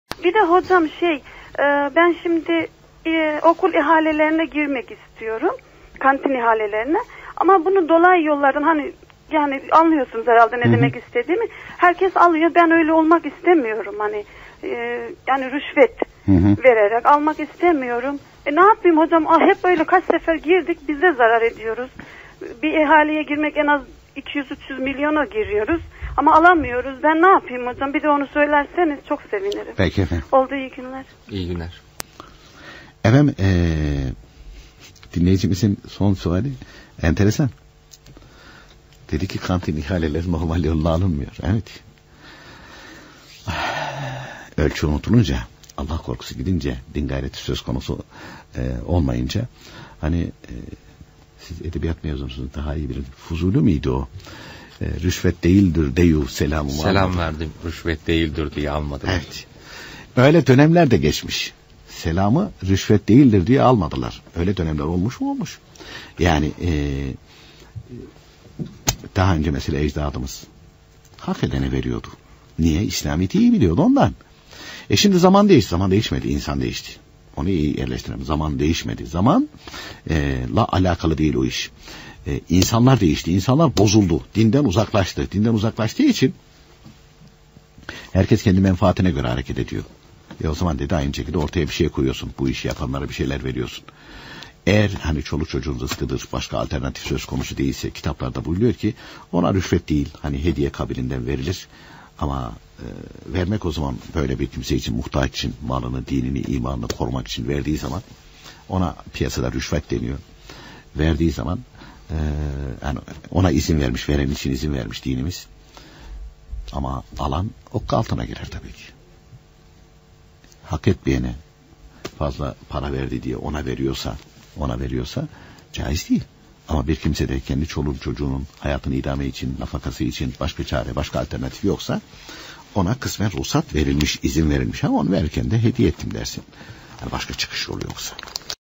Kantin ihalesine girmek istiyorum ama herkes rüşvet vererek alıyor ne yapayım? | Osman Ünlü hoca
Okul kantin ihalesine girmek istiyorum herkes rüşvet veriyor bende vermek istemiyorum?
Okul kantin ihalesine girmek istiyorum ama normal yolla almıyorum ne yapayım?